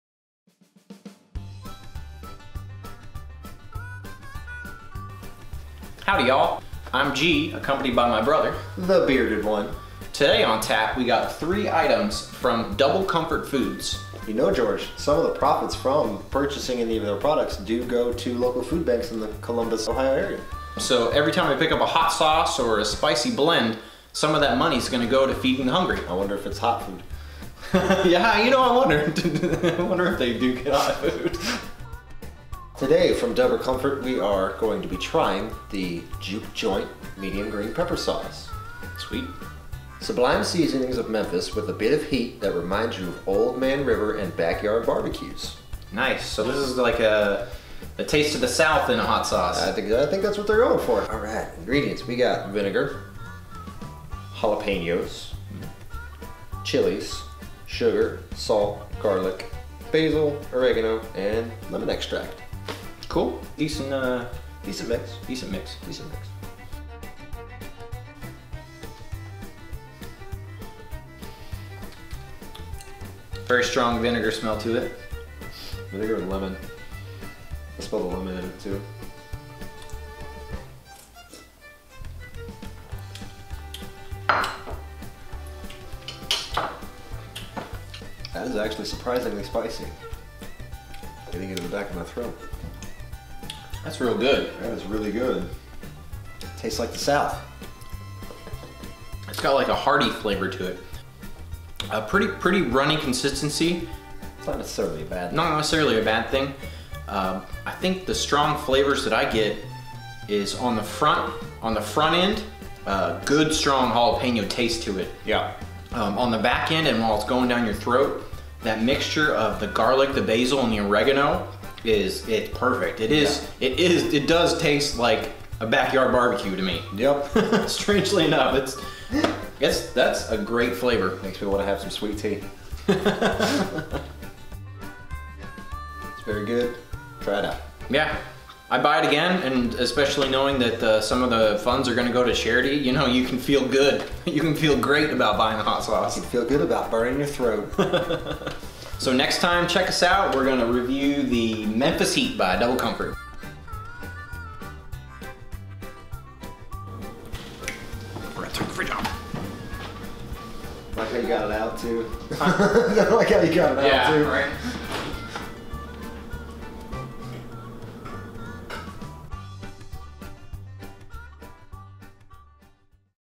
Juke Joint Hot Sauce Review - Double Comfort Foods - DNTDYE
G and The Bearded One sample a Double Comfort Foods hot sauce - Juke Joint.  Special thanks to the folks over at B Customs for providing our DNT DYE Sign.